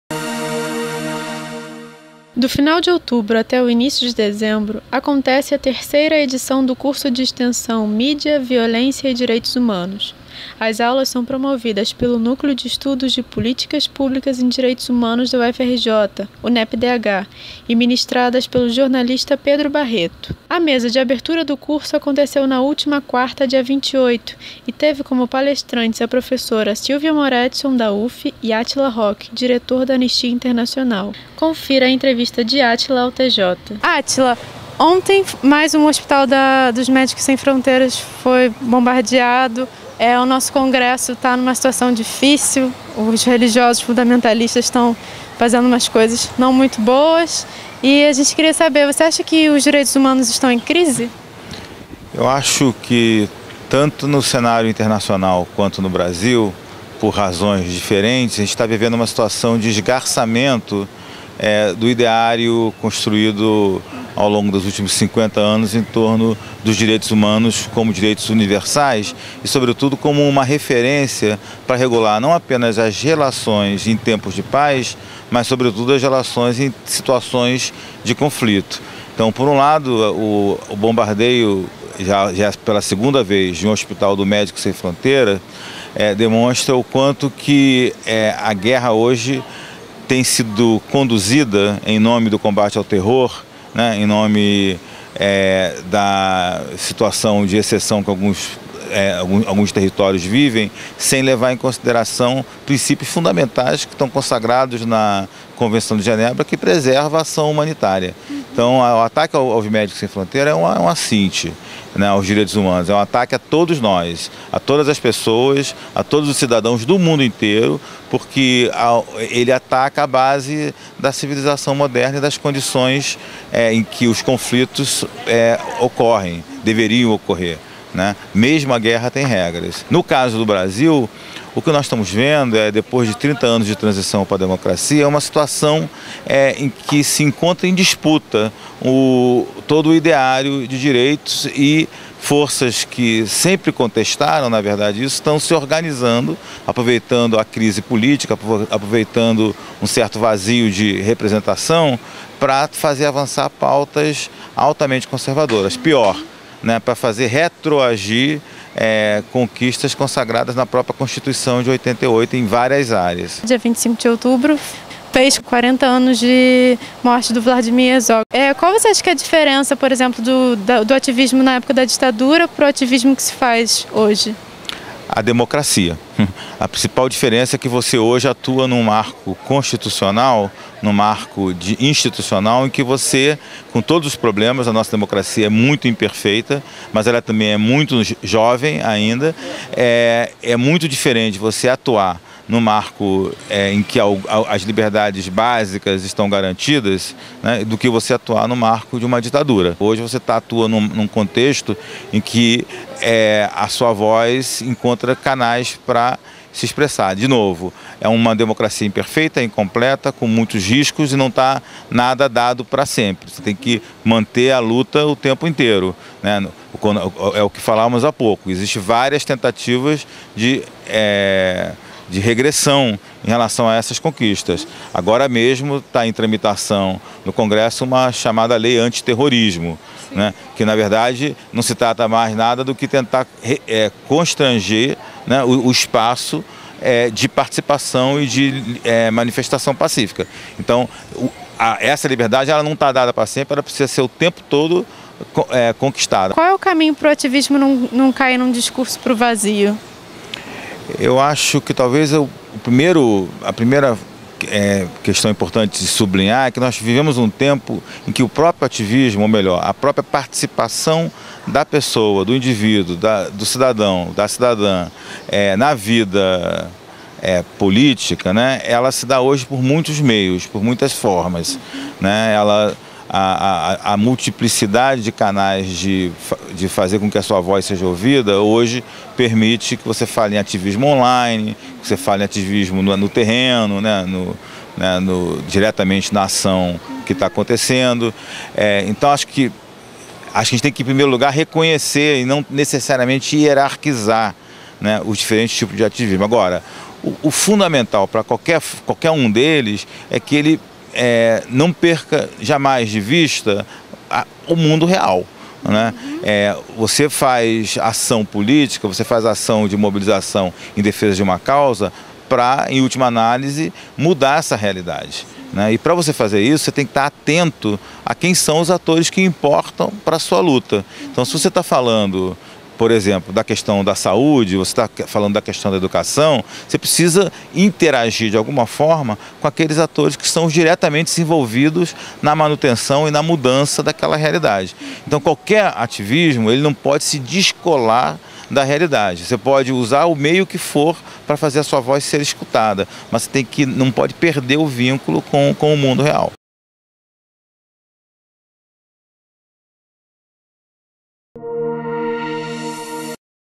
Átila Roque, diretor da Anistia Internacional, fala sobre Direitos Humanos e ativismo
Na última quarta, dia 28, aconteceu no campus da Praia Vermelha a mesa de abertura do curso de extenção Mídia, Violência e Direitos Humanos, que contou com a professora Sylvia Moretzsohn, da UFF, e Átila Roque, diretor da Anistia Internacional.  O curso é promovido pelo Núcleo de Estudos de Políticas Públicas em Direitos Humanos da UFRJ, o Nepp-DH, e as aulas serão ministradas pelo jornalista Pedro Barreto.